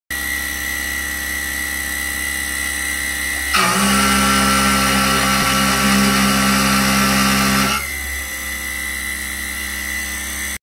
Rougher test 1
Lakeshore carbide .250" rougher @ 2500rpm 25ipm 30% width 80% depth